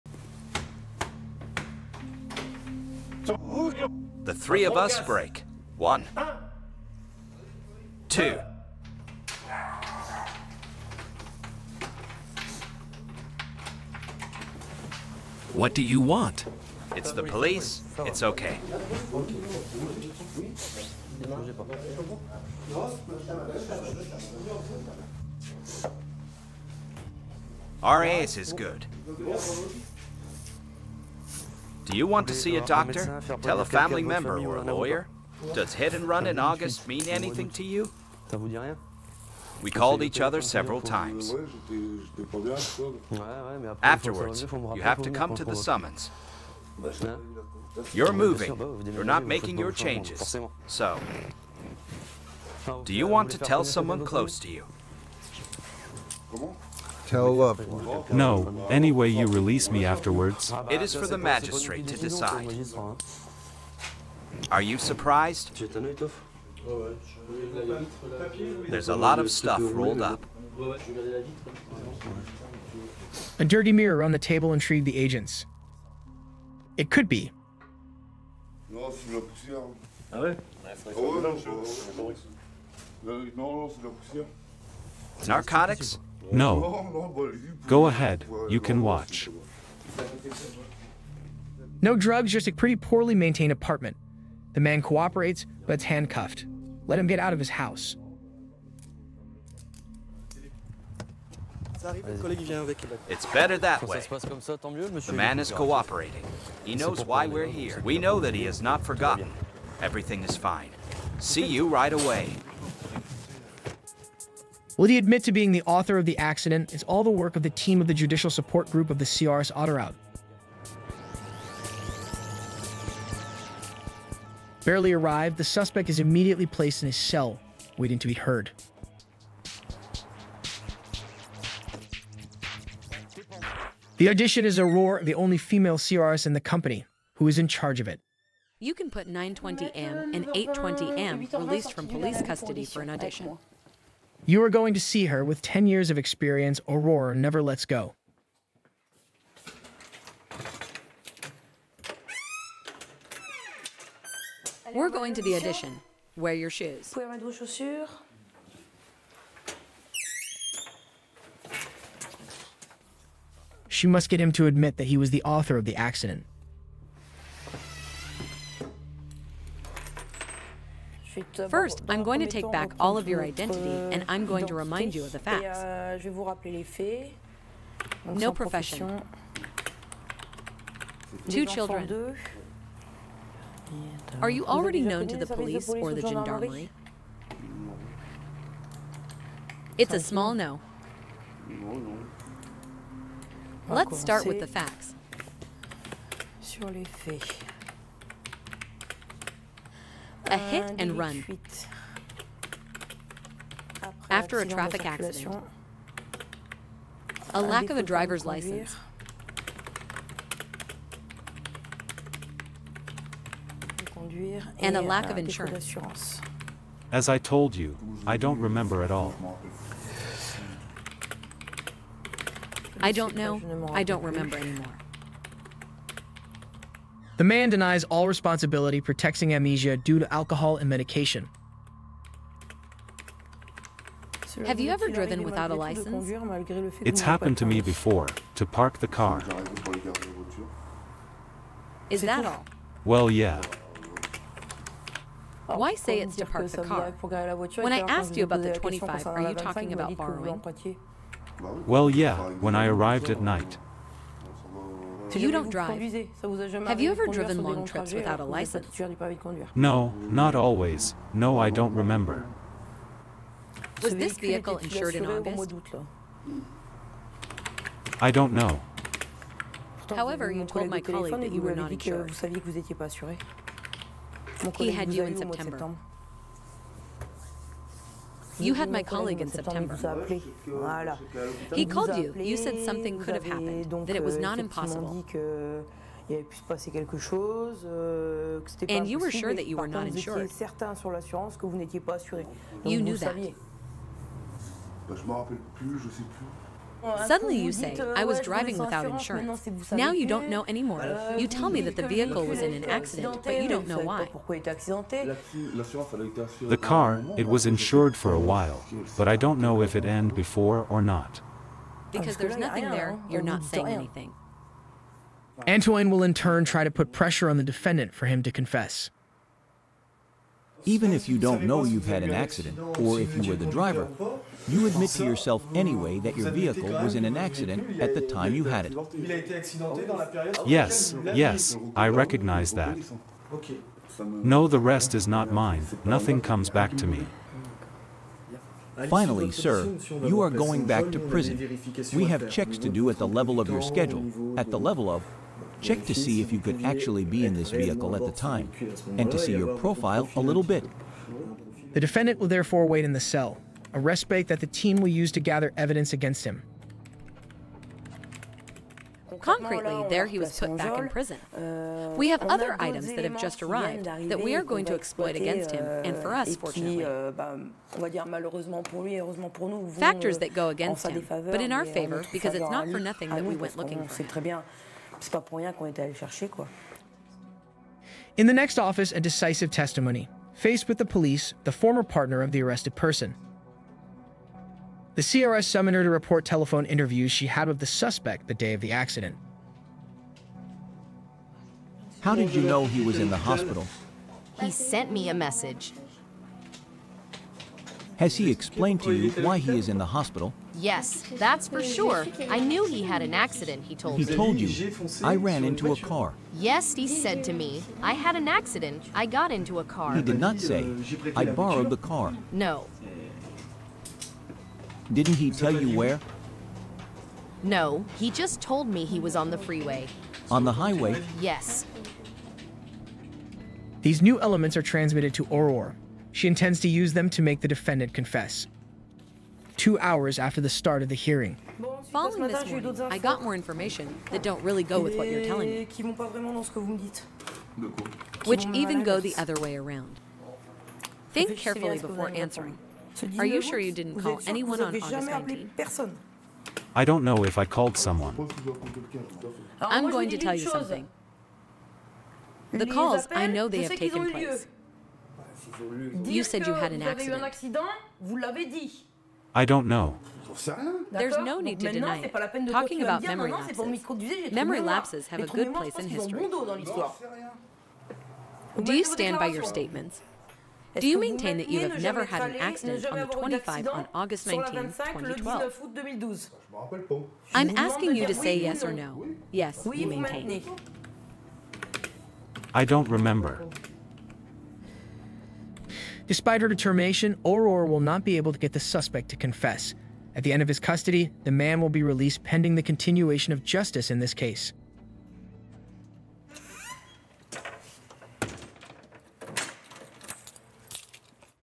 Police arrest & interrogation
Lead suspect in a hit & run charge is arrested and interrogated